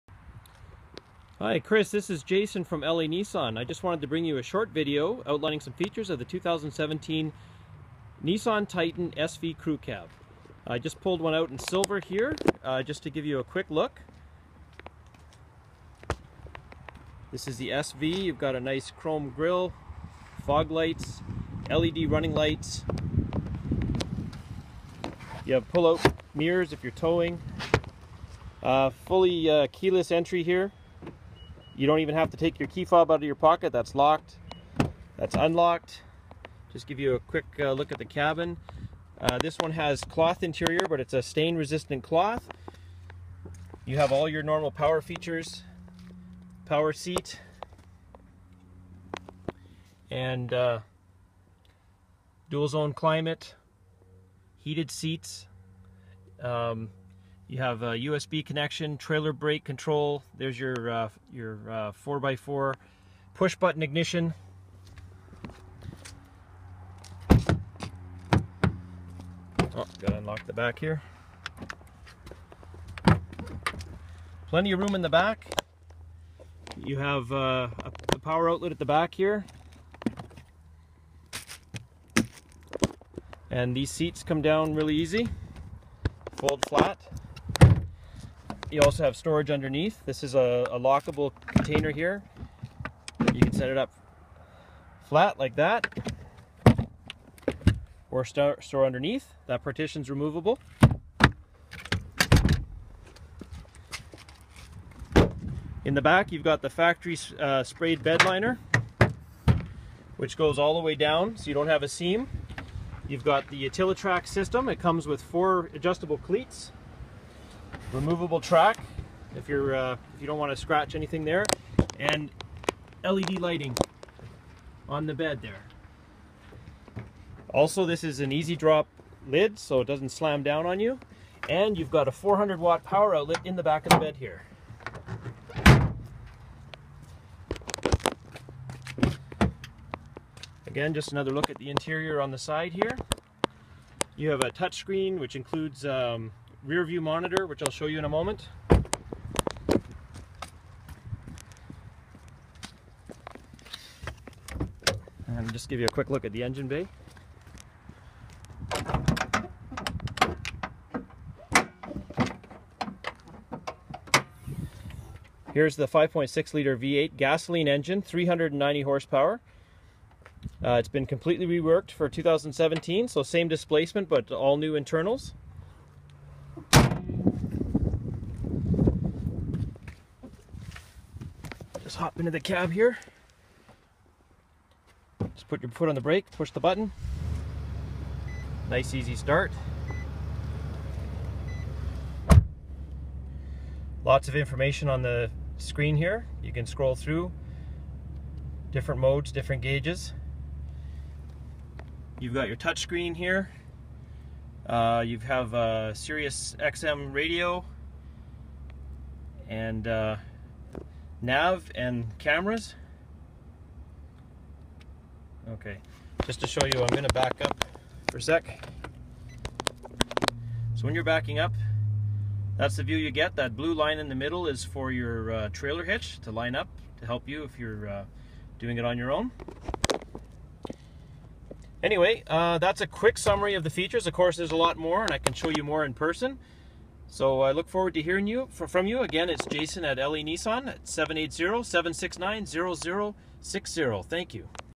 A Titan for Chris!
Here's a brief walkaround of the 2017 Nissan Titan SV Crew Cab.   Please bare with me as I'm new to doing videos like this and didn't have a cameraman handy to help.